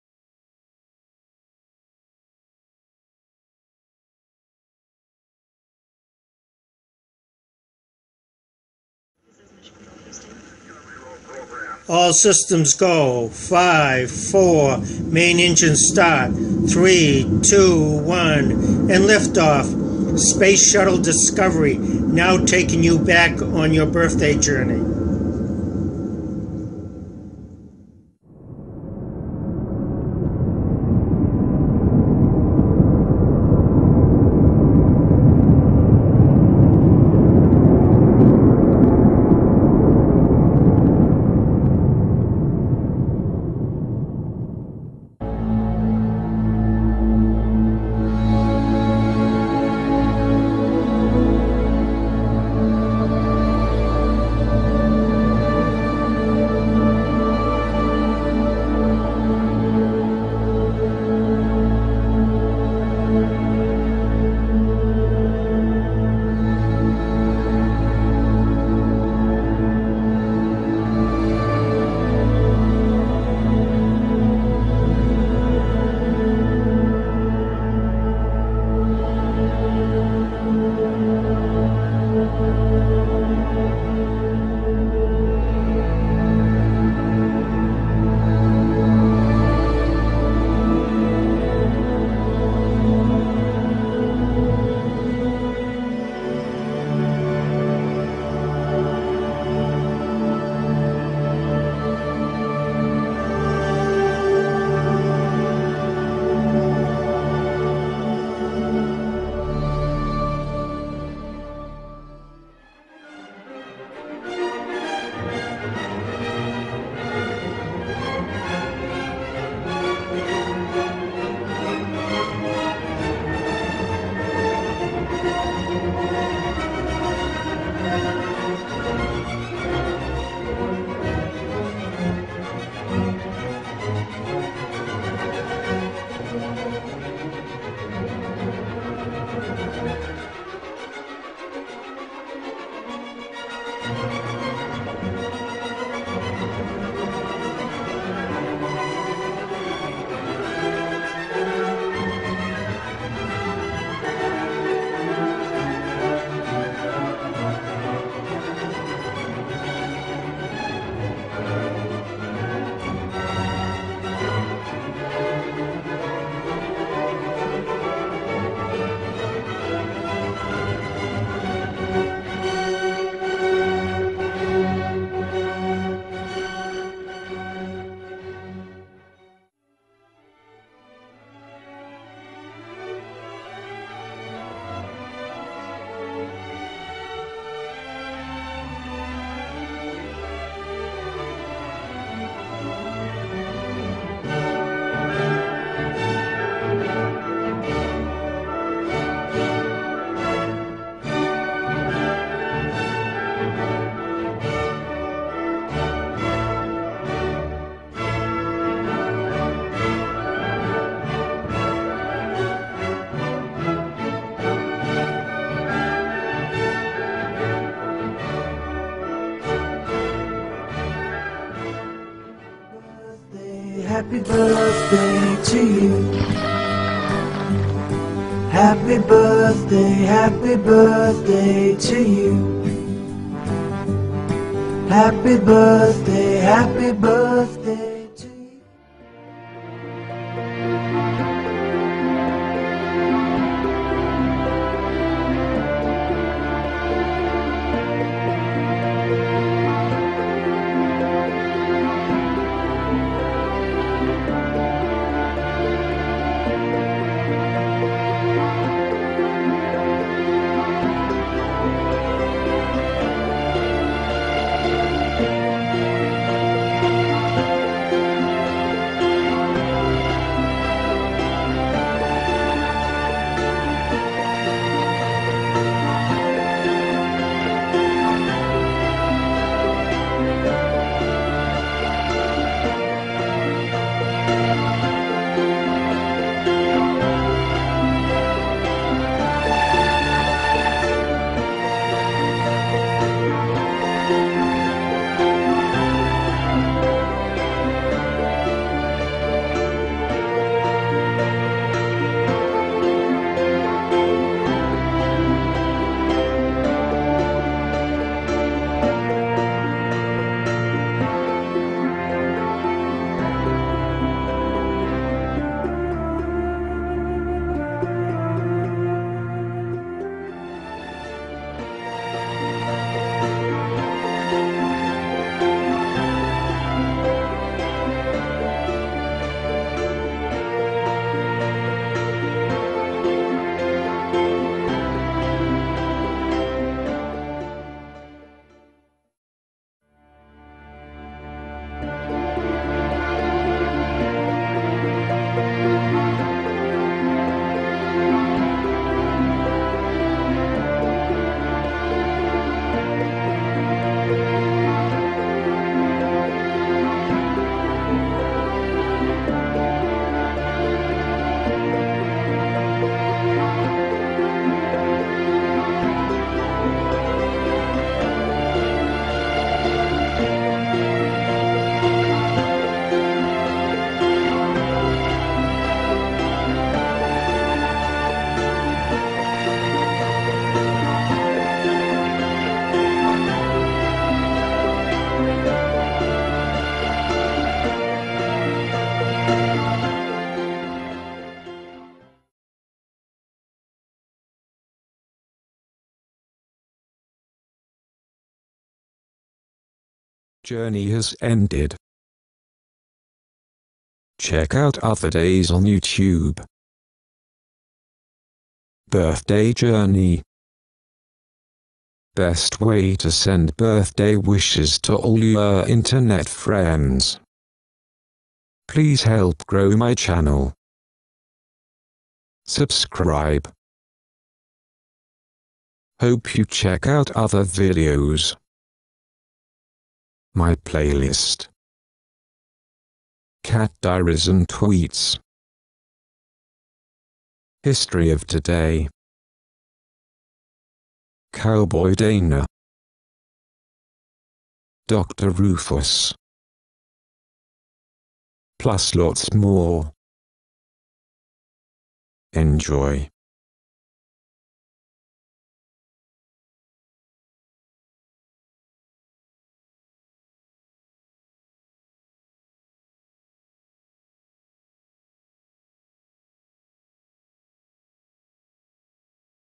Birthday Journey May 29 (music)
Birthday Journey- FREE --Celebrate your Birthday May 29 through time. Journey through historical events, see your horoscope and famous people who share your Birthday day. GREAT way to send Birthday wishes to all your internet Friends!!! FREE and easy--just copy and paste